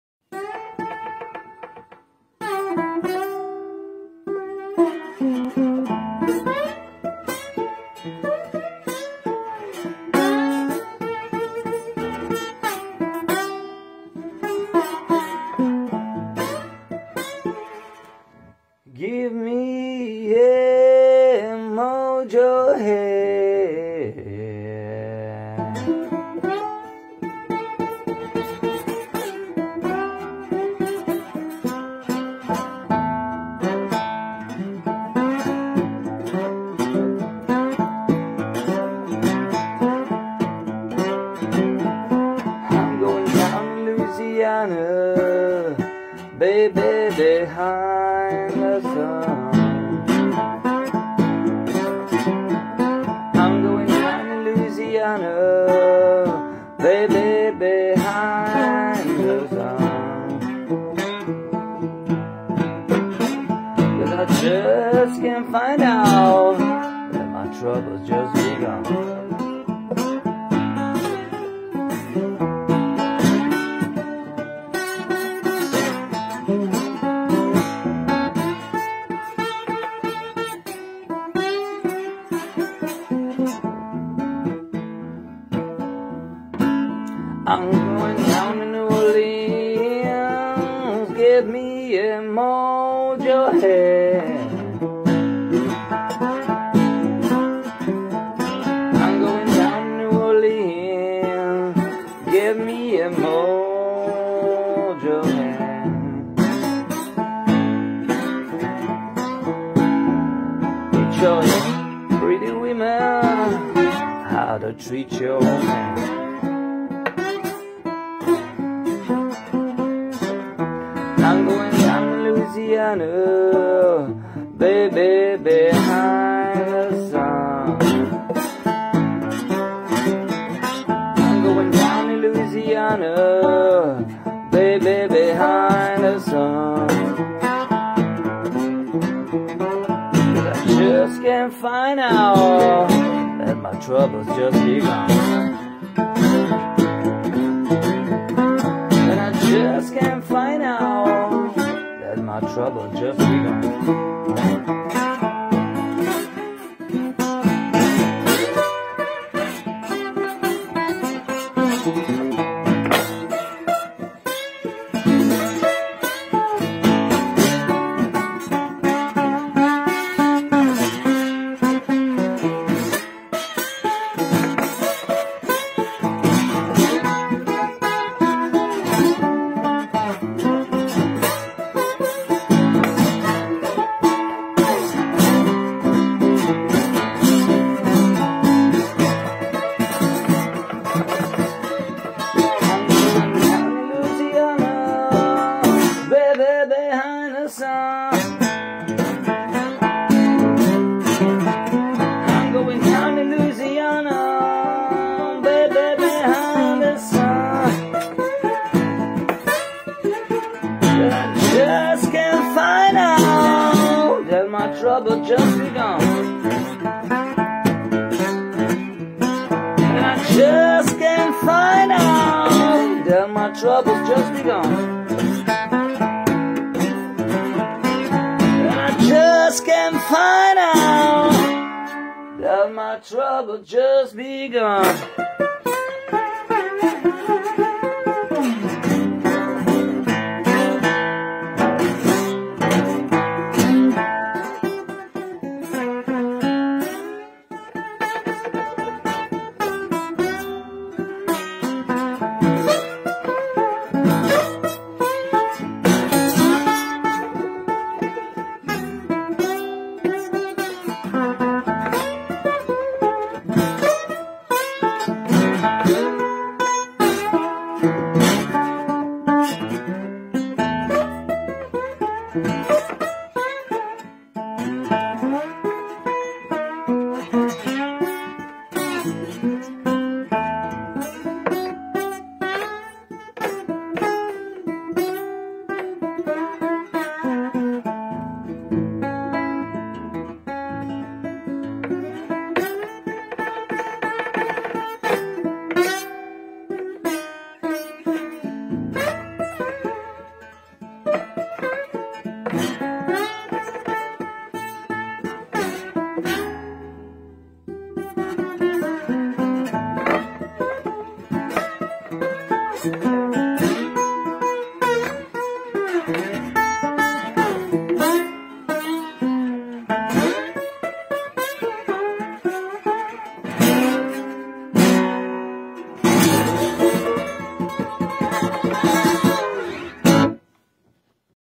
Matteacci's Resobelle resophonic Guitars.
questa chitarra era una normale chitarra acustica trasformata in chitarra resofonica.
il risonatore in acciaio verniciato è stato ricavato da un coperchio di tegame oppornamente decorato.
 Il disegno sulla chitarra è inciso con il laser.Mr. Luca Semeria players.